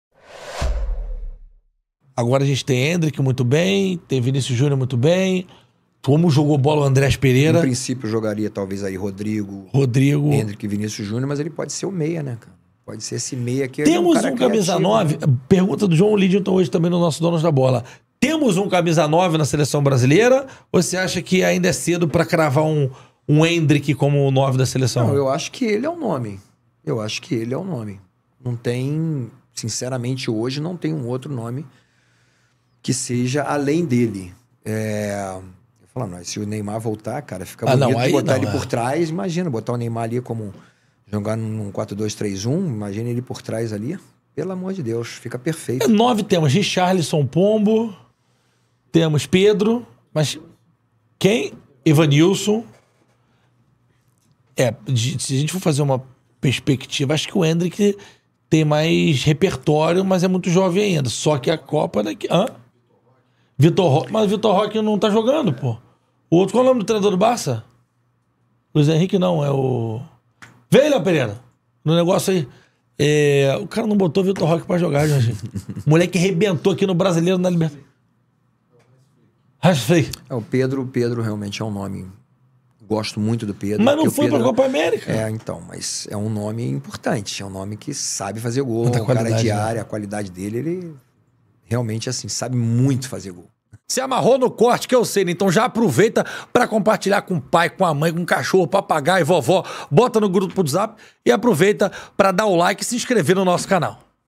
ENDRICK VAI SER O CAMISA 9 DO HEXA?
O ex-jogador da Seleção Brasileira Jorginho, campeão do mundo em 94, falou sobre quem deve ser o centroavante camisa 9 do Brasil na próxima Copa do Mundo e o favoritismo do garoto Endrick para assumir a vaga.